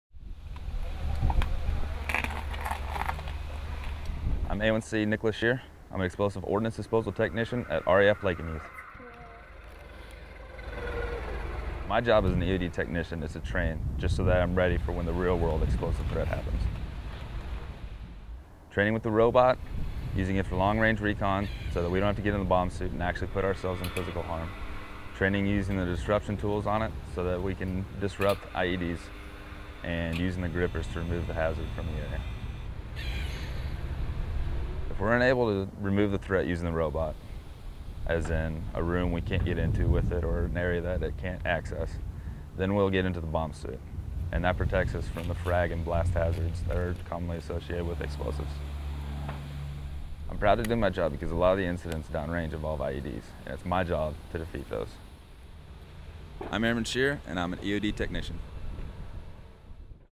Defusing Situations
Explosive ordnance disposal technicians train to go downrange. An Airman at RAF Lakenheath tells us how he does it.